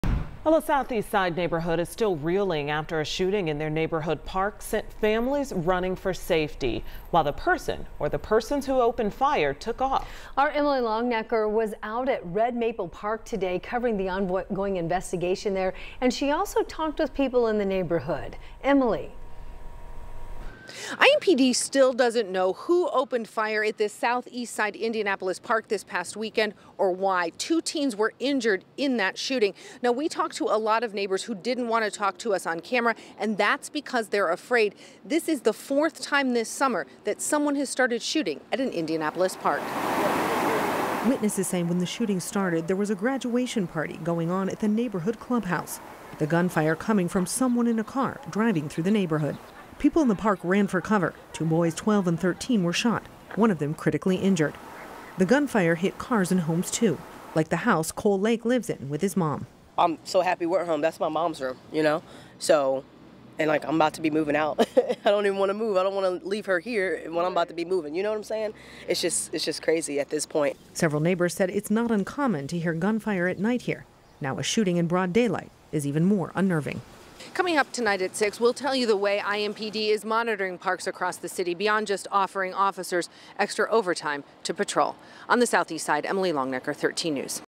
Shooting at Red Maple Park sends families running for cover
A neighborhood on the southeast side of Indianapolis is reeling after a shooting in their community park.